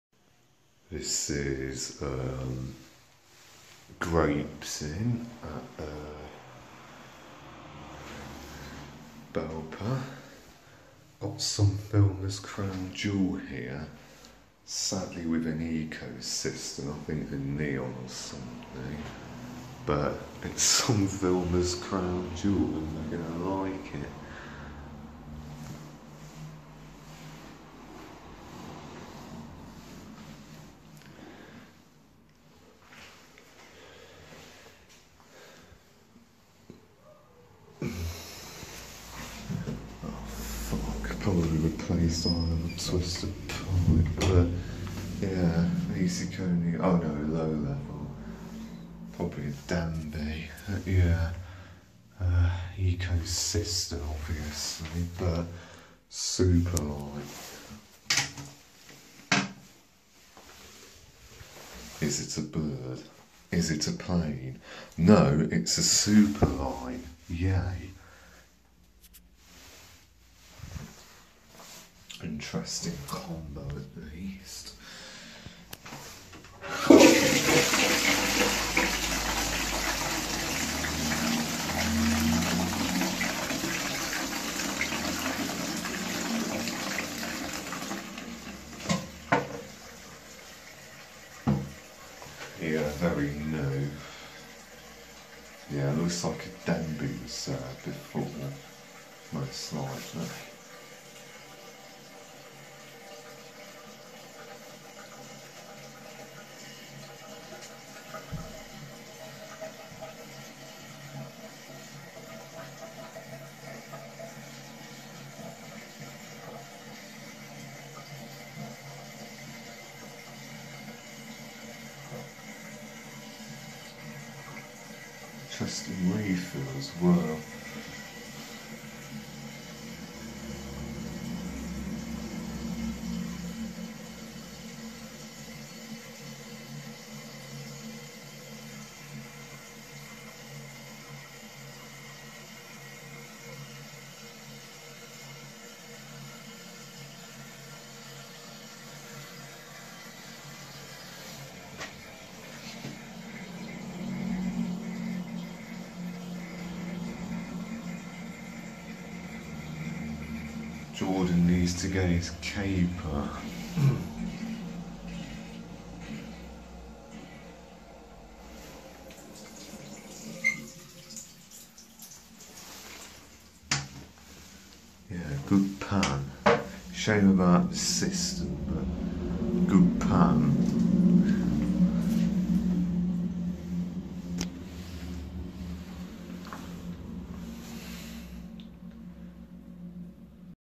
Shires Supaline/LLPB LN, PH, Belper (4)
This is in another Belper PH.

Very unusual combo here. Good pan, a Shires Suplaline pan with a LLPB Neon cistern, which appears to be very recent, due to the sticker on it. Not the first LL Neon found by one of us lot either.

Overall Gents Grade: C